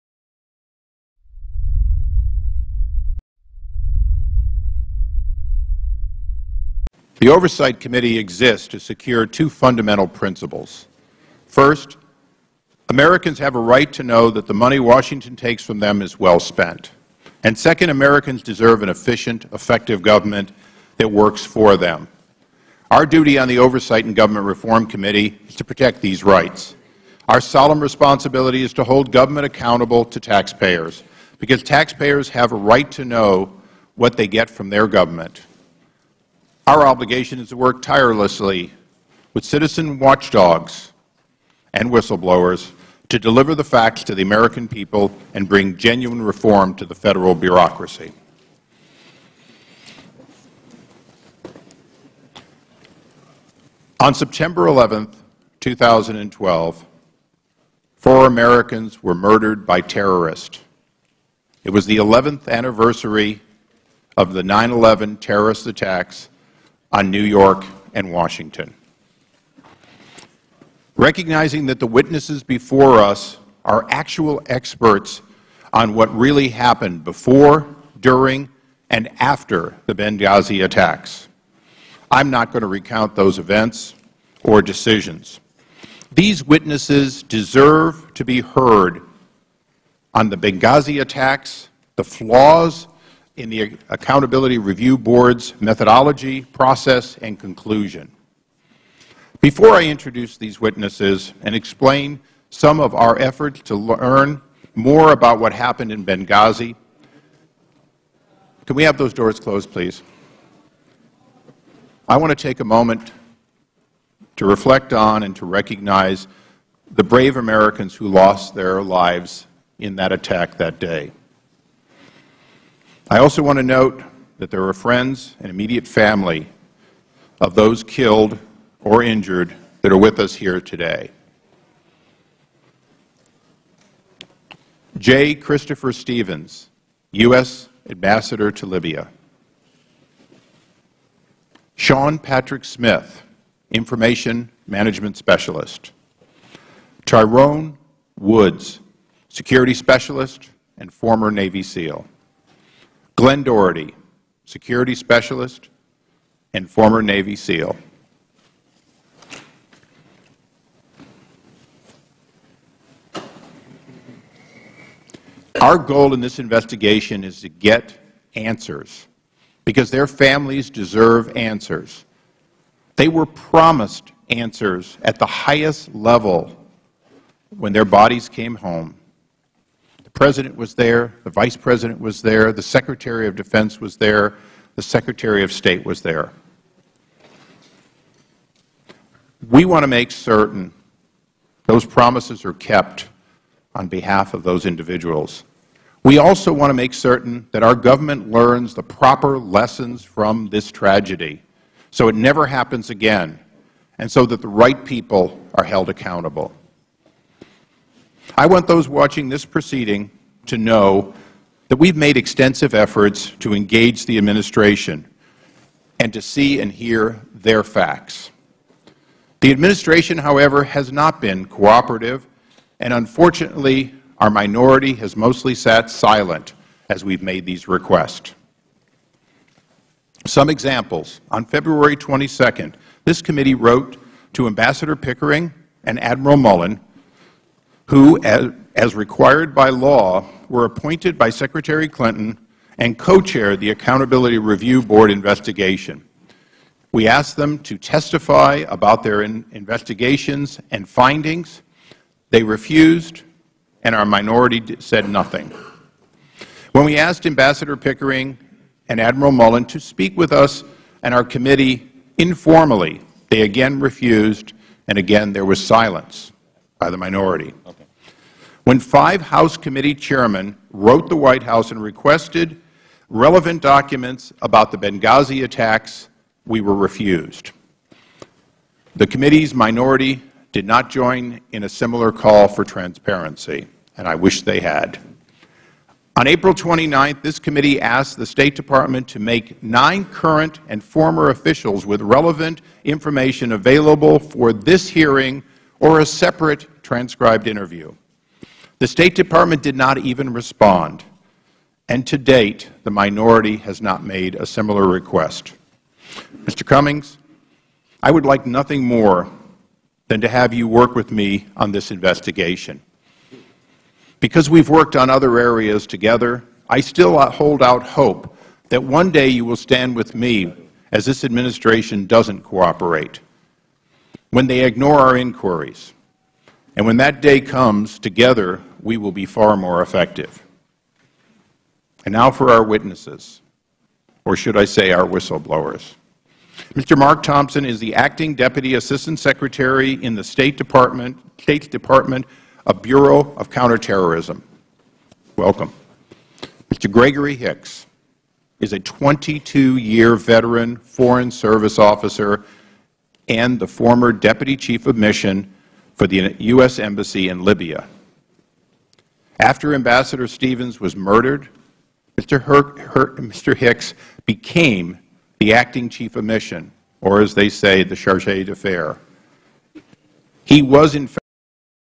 Whistle Blowers Before House Oversight and Government Reform Chairman Darrell Issa Speech
The stand down order came from Kennedy and Clinton. Help was 2-2/12 hours away. This noes not account for help never asked for to NATO or UN or Italy that was near by or any of our other allies. Libya refused to send help. Ambassador Chris Stevens was sent to a hospital in control by the same group of militia that attacked the embassy.
They knew days ahead of the attack it was coming and asked for help but none was send and no evacuation was ordered. I had nothing to do with any film but was a premeditated planned attack. there was four waves of attacks.
Where are the attackers bodies?